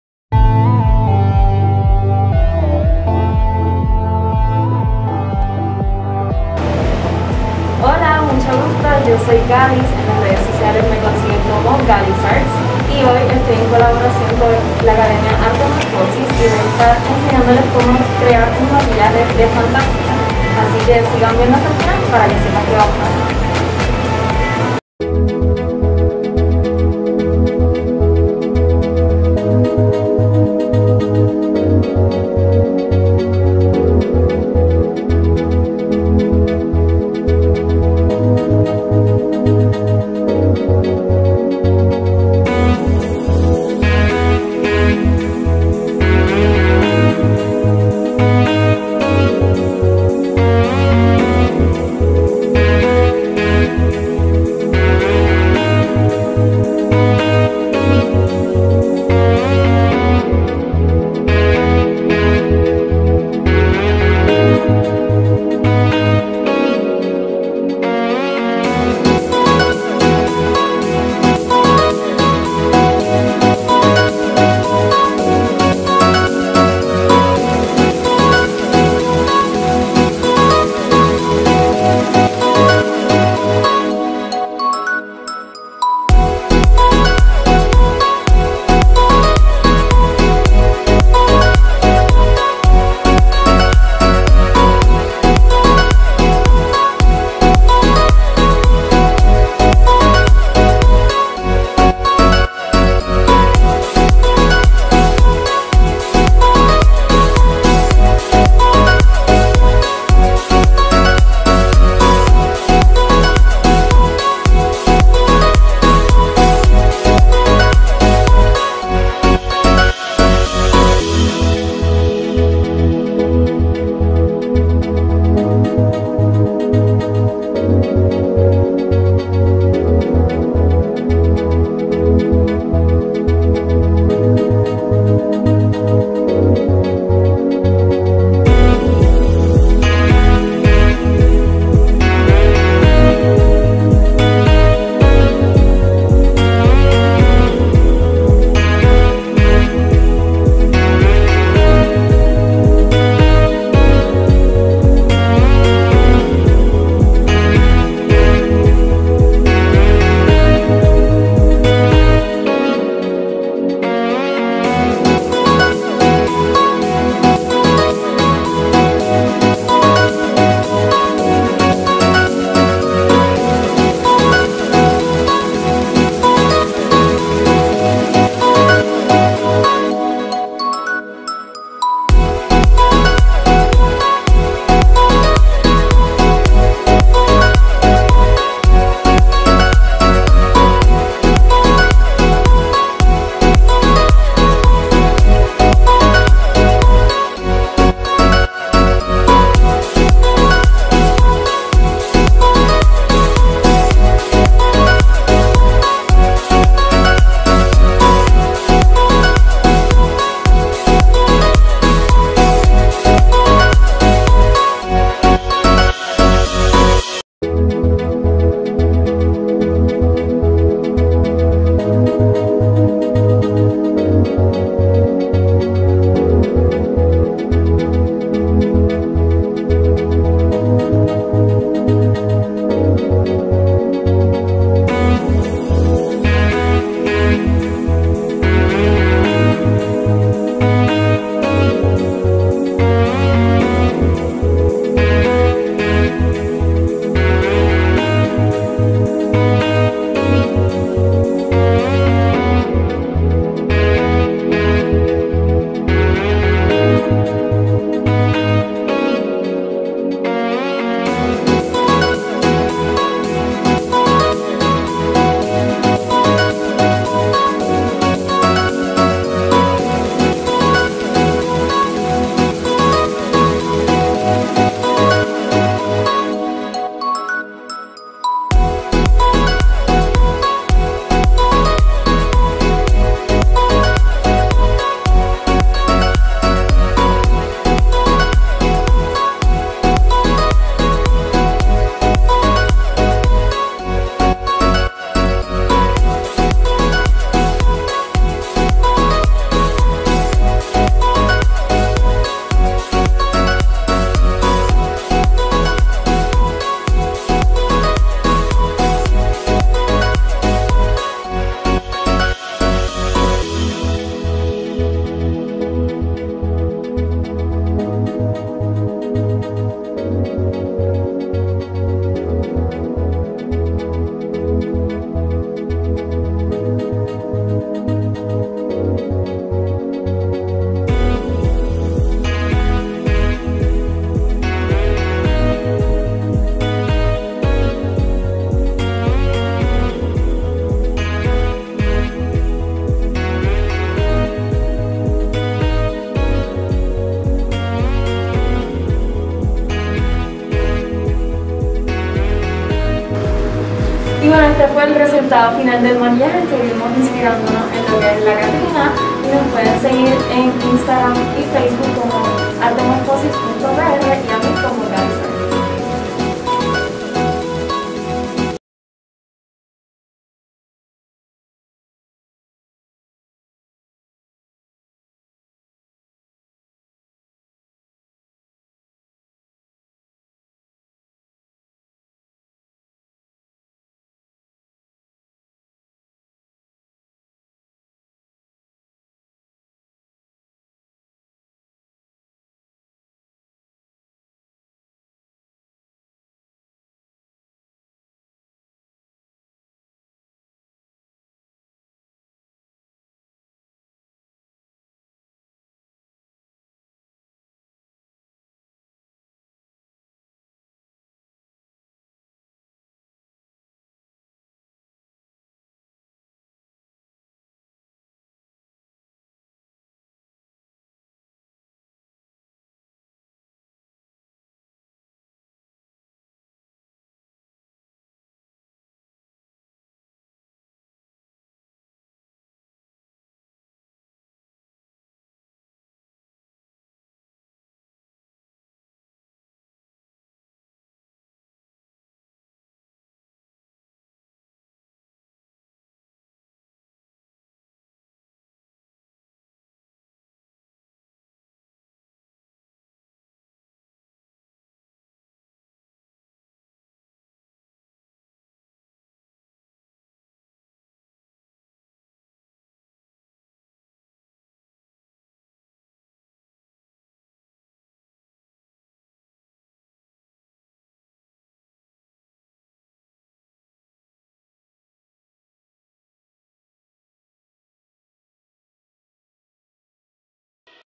Halloween Makeup Tutorial
🎃Halloween Trick or Treat 🎃

Te presentamos nuestro tutorial de maquillaje de fantasía para este Halloween realizado  por la talentosa @galisarts .

Déjanos saber en los comentarios qué te pareció este increíble maquillaje.

Modelo: @thaliaruby
Video: @elynel.photo
Edición: @thebuckettrips
Producción: EQUIPO ARTEMORFOSIS TV